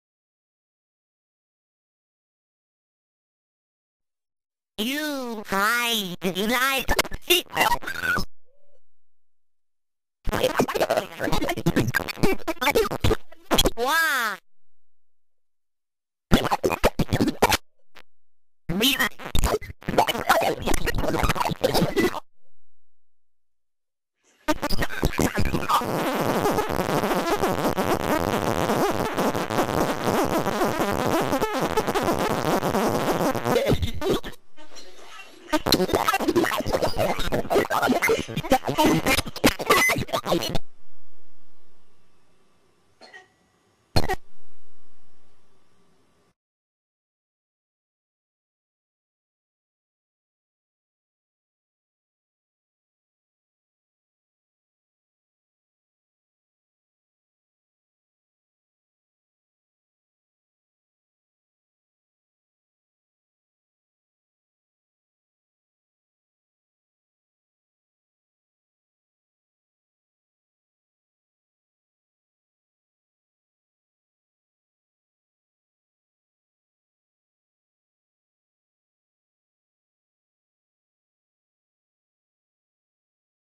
CFAI teaching grant examples
This short video shows 2 examples of projects completed during the last phase of my CFAI-grant period.